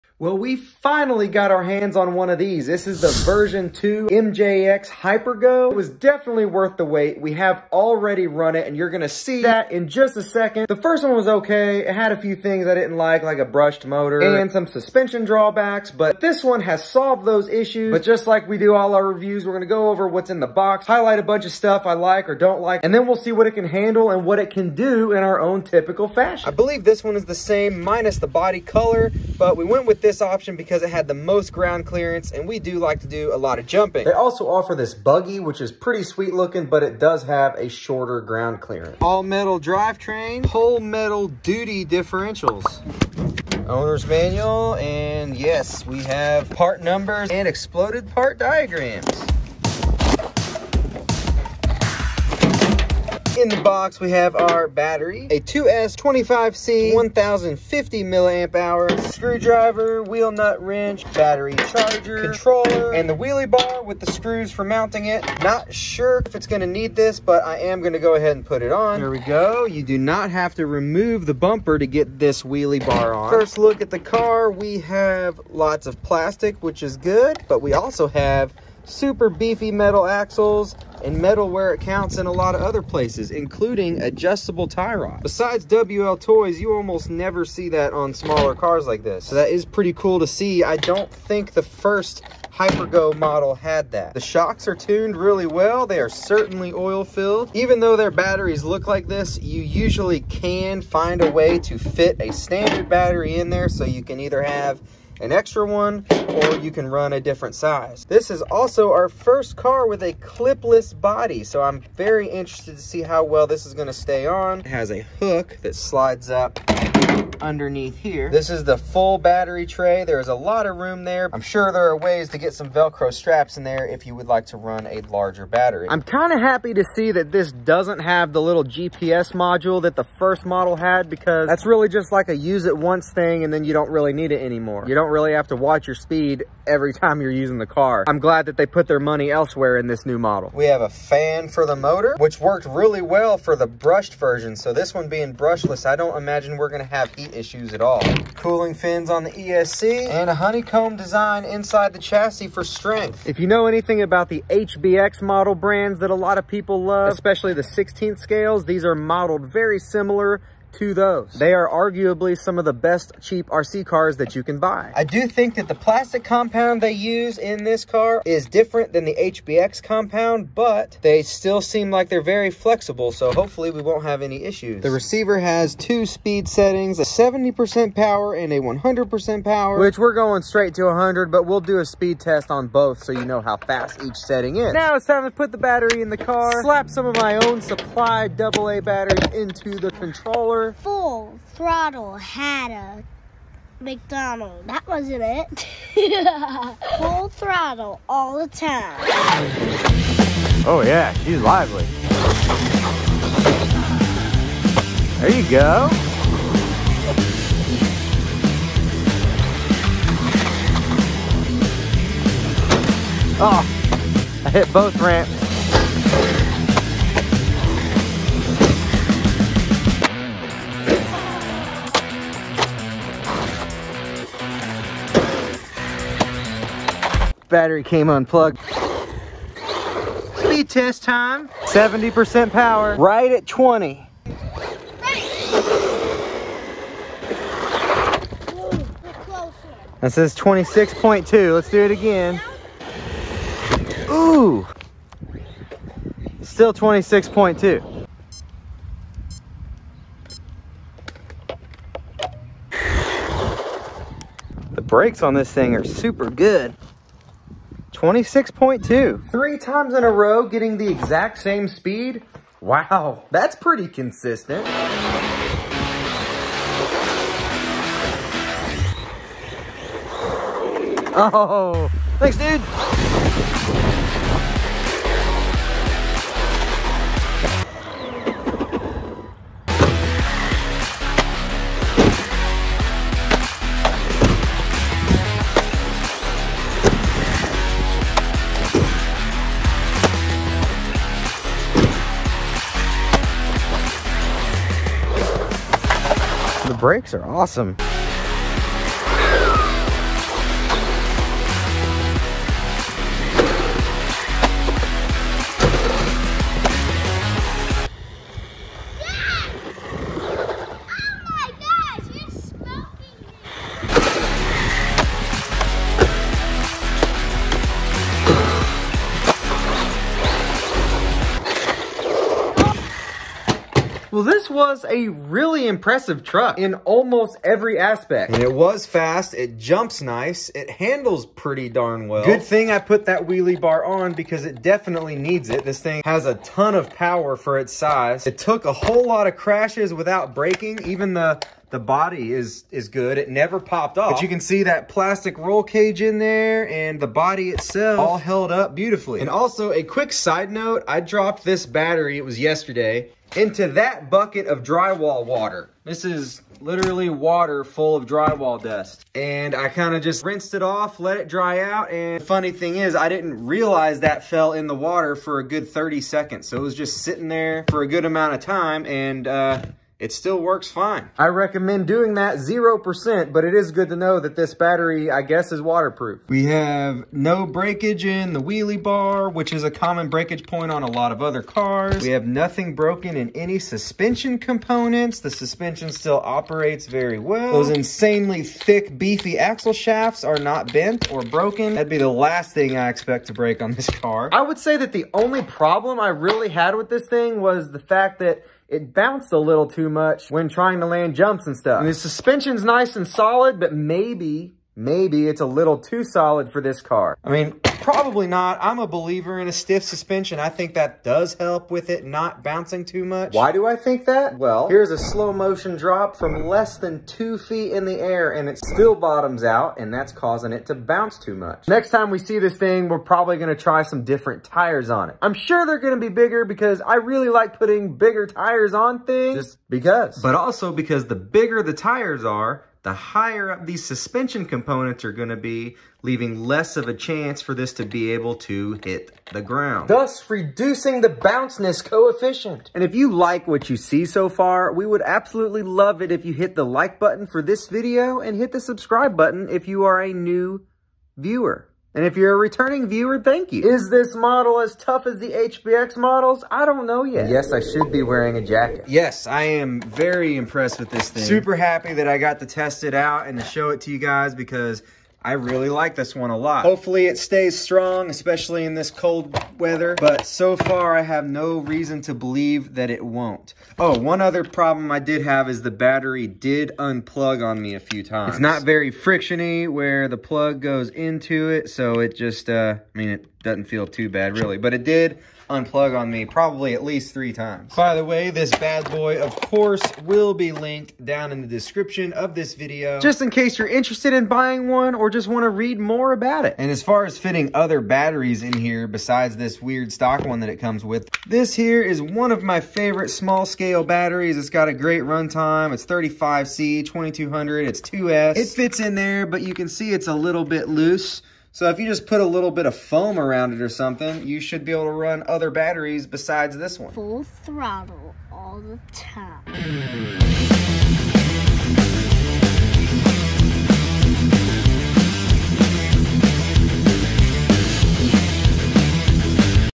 New EXTREMELY FUN RC car! MJX HYPER GO v2
Ovonic Battery shown near the end of the video

The HBX 16889a pro is amazing, but this may be the most fun I’ve had with a 1:16 scale.
It’s even fun to use the brakes!
Can’t wait to try a few new thing with it to improve it even more